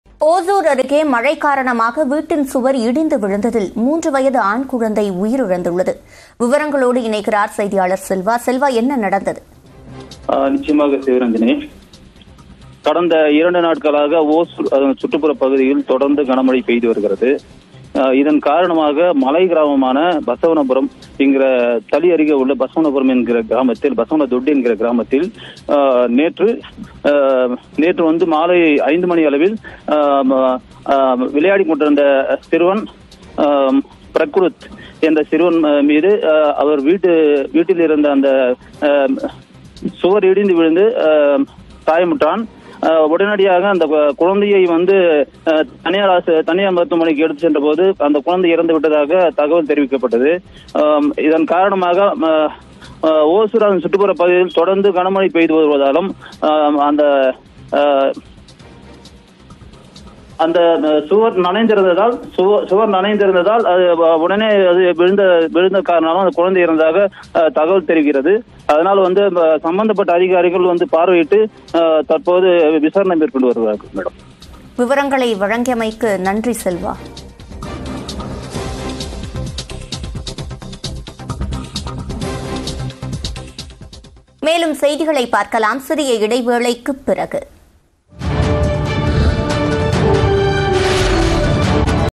JUST NOW | தொடர் மழையால் இடிந்த ஓட்டு வீடு - 3 வயது குழந்தைக்கு என்ன ஆனது? | Tamil News
தொடர் மழையால் இடிந்த ஓட்டு வீடு - 3 வயது குழந்தைக்கு என்ன ஆனது?

News18TamilNadu | Hosur | Rain | Disaster | House Destroyed | Ceiling | Tamil News